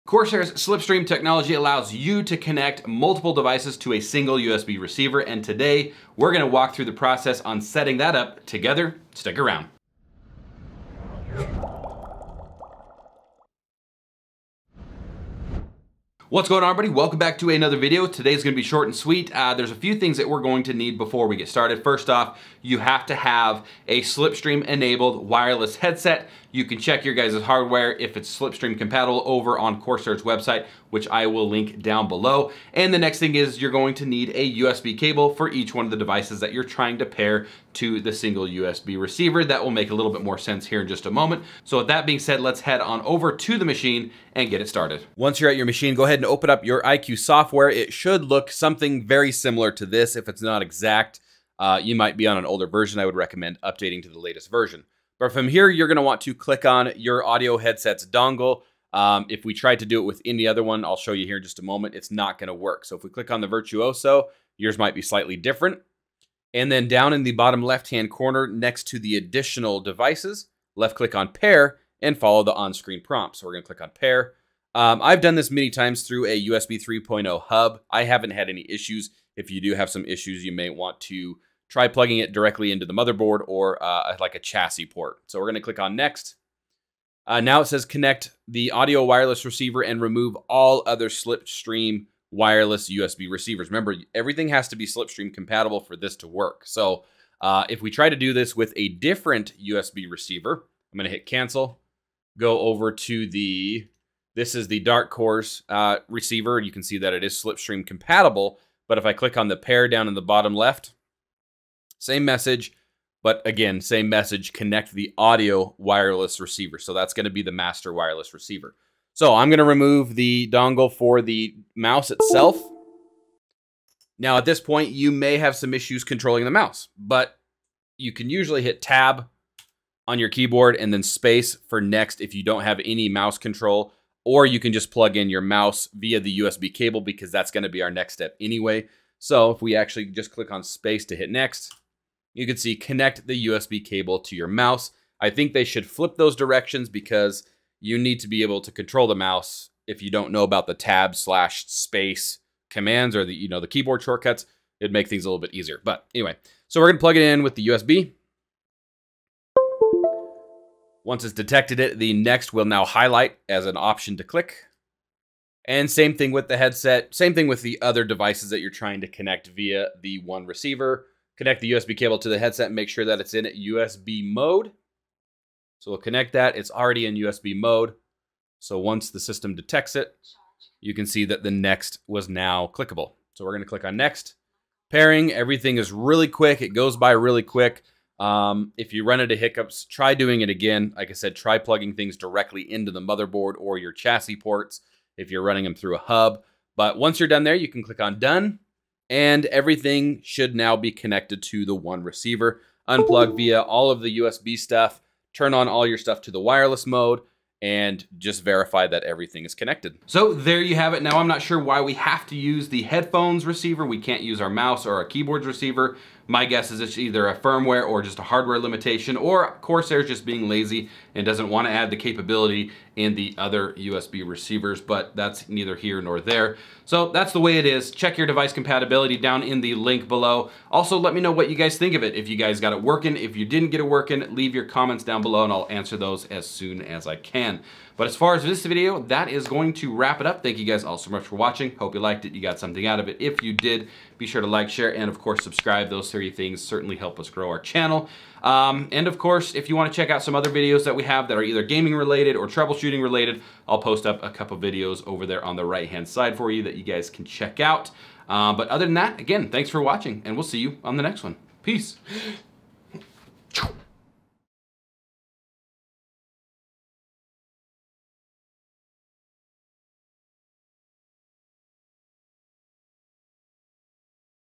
Connect & Control Multiple Corsair Slipstream Devices with ONE USB!
SLIPSTREAM is Corsair’s answer to the challenges that have traditionally led competitive gamers to avoid wireless peripherals. It delivers super fast response times and reliable, lag-free connections to satisfy even the most demanding of gamers. And the good news is that it’s accessible for people of all budgets, with SLIPSTREAM-enabled products. In this video, we show you how to connect multiple Corsair SLIPSTREAM devices to one USB dongle.